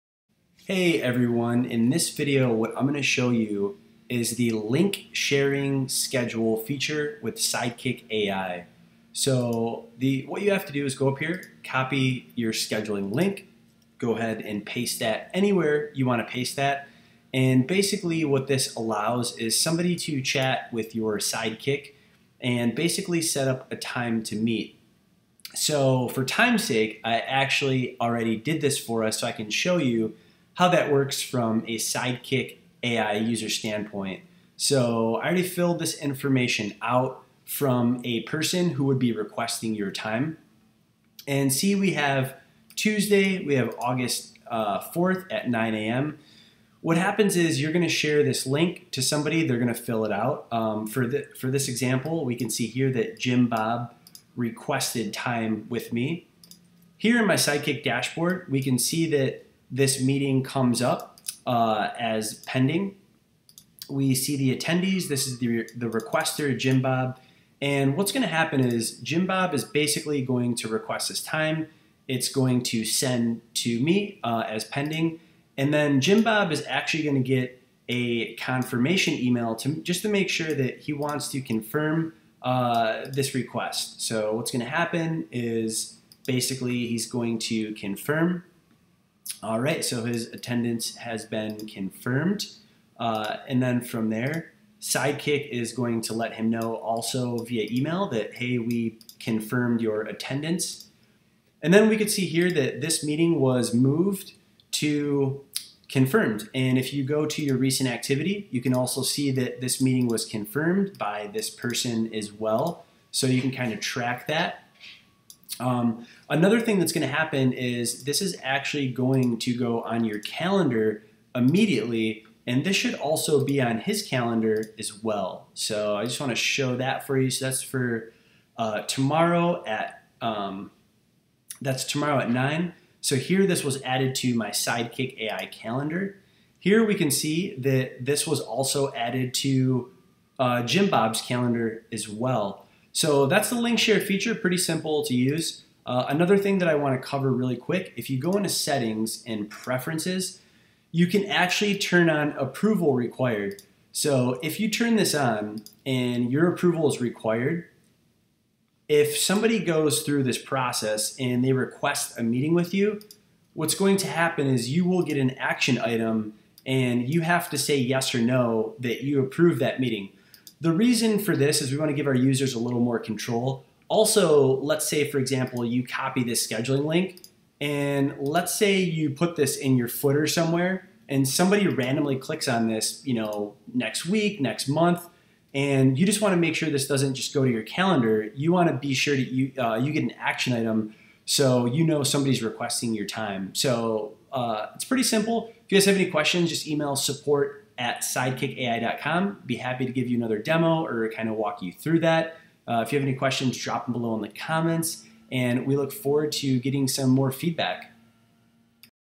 Link Share Scheduling - Sidekick Ai
Sidekick Ai gives users the flexibility to schedule meetings in a few ways. One way is through Link Share Scheduling. Simply share your Sidekick Ai link with someone and they can chat with your Sidekick to book a meeting with you.

Our scheduling software currently offers four unique ways to schedule a meeting for users:

4 - Auto Scheduling